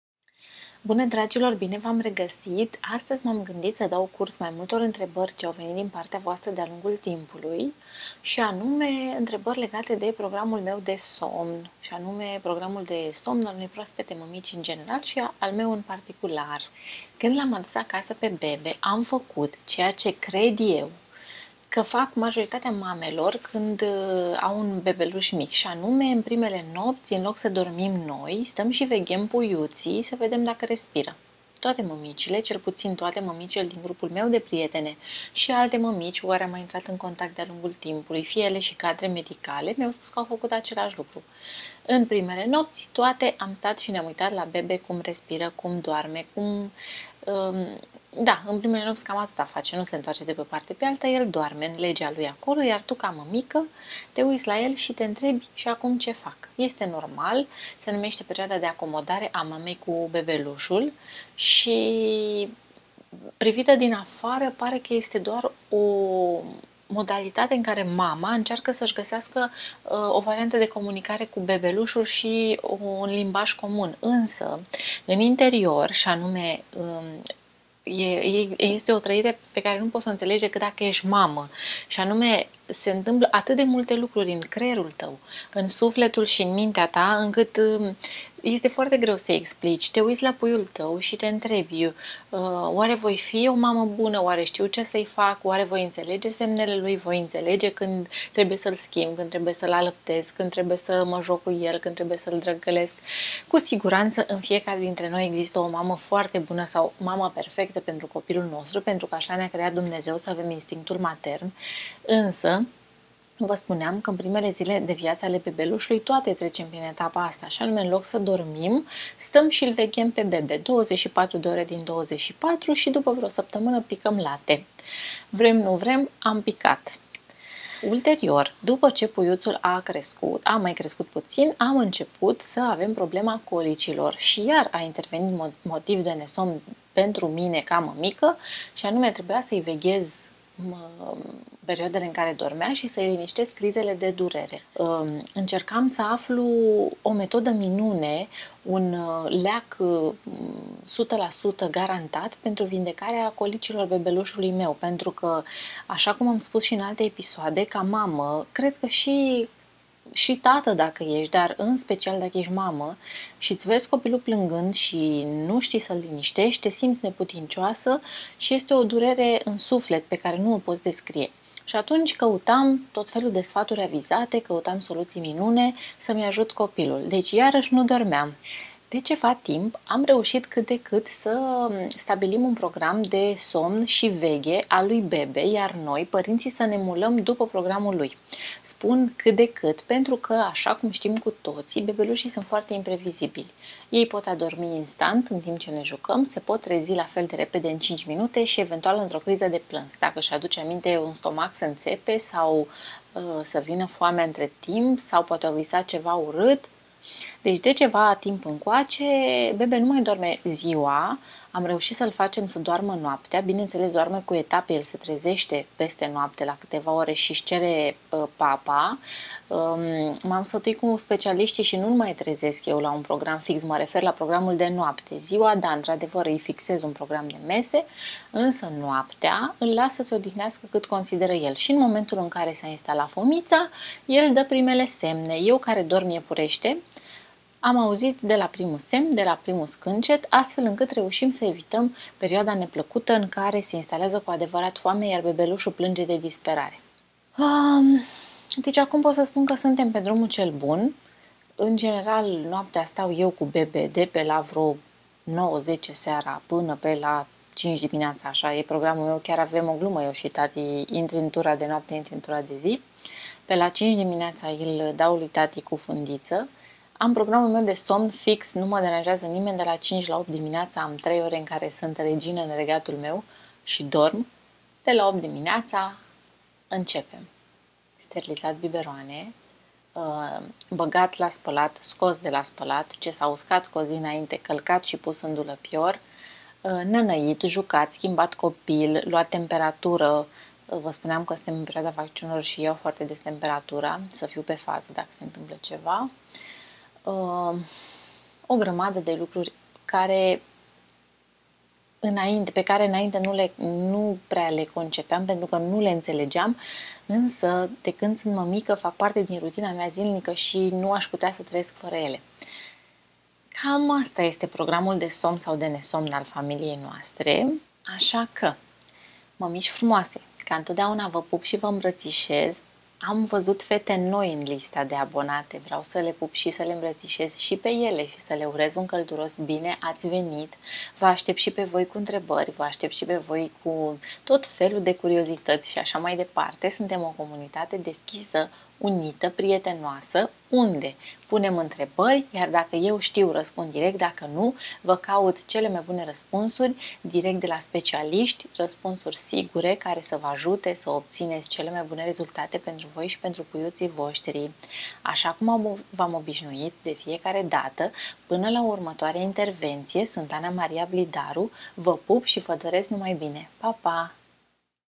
Ce faci cu somnul când vine bebe acasă
Ce faci cu somnul? Bună întrebare... În primele nopți te uiți la bebe cum doarme, simți nevoia să-i asculți respirația și să te asiguri că totul e în regulă. Numai că somnul tău, ca mamă, se duce pe apa sâmbetei. Ce soluții ai? Vedeți în filmuleț la ce am recurs eu!

Mulţumim!